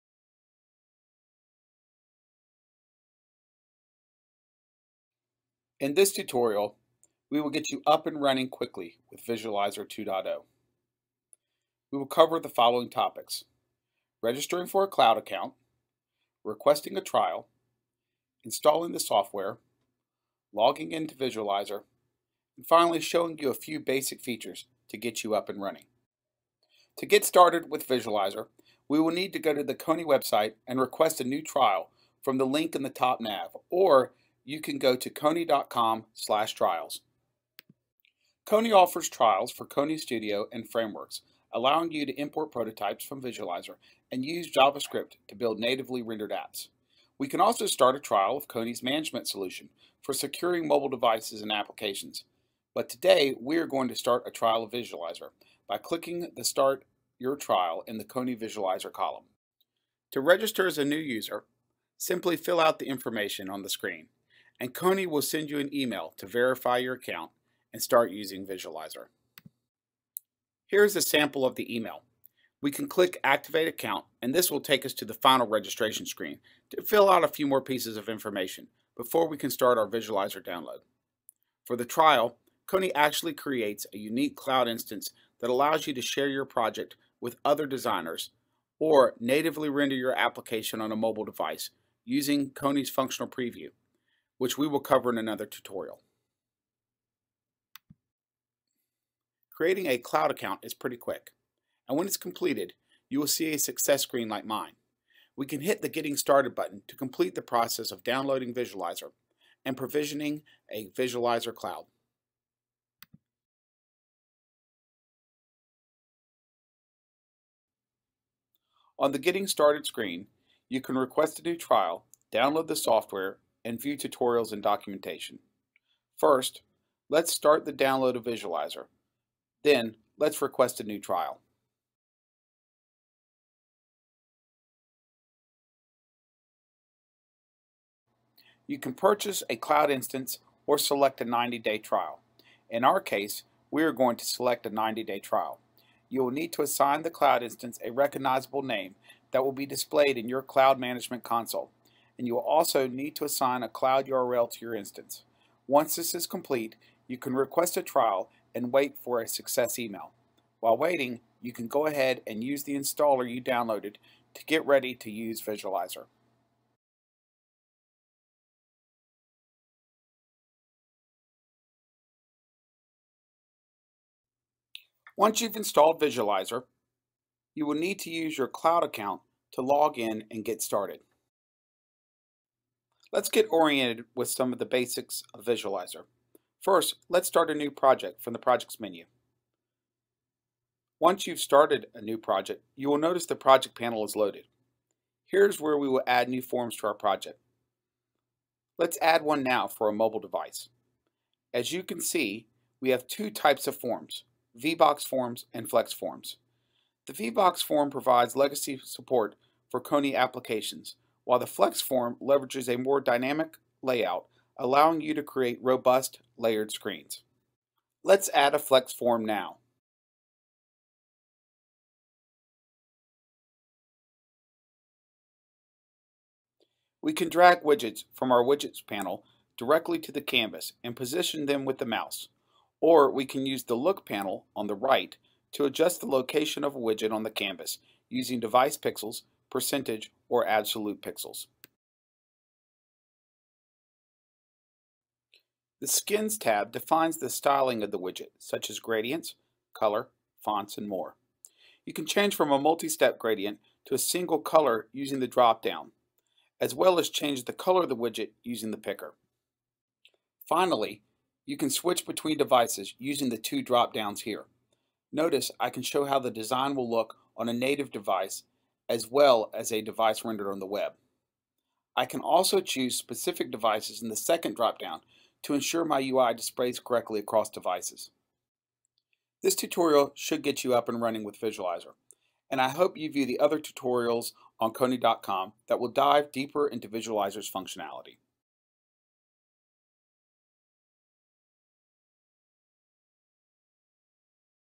Getting Started with Volt MX Iris
This is the first in a series of Volt MX Iris videos that help enable designers create robust prototypes that can be imported directly into Volt MX Studio.